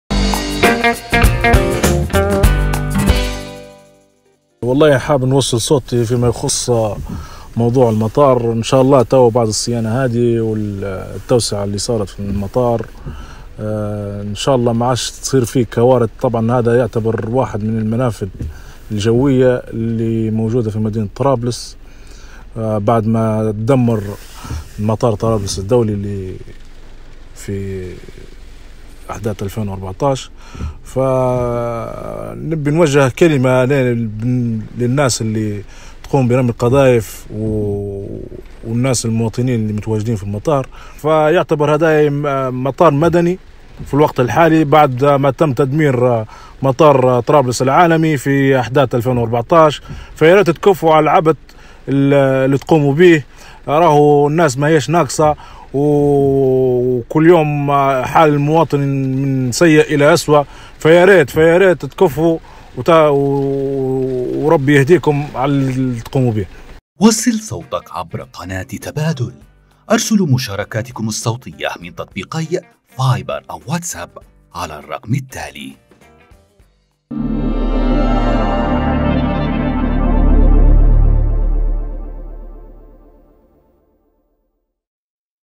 مع إعادة إفتتاح مطار معيتيقة..هل سيخرج المطار من دائرة التجاذبات؟
وصل صوتك